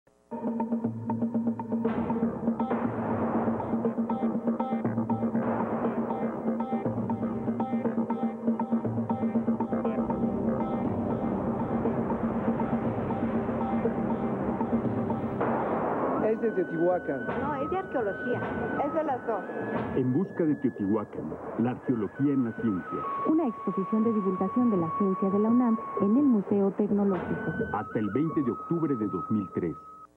Comerciales mexicanos: UNAM-MUTEC 2003
Exposición En busca de Teotihuacan la arqueologia de la ciencia en el MUTEC por la UNAM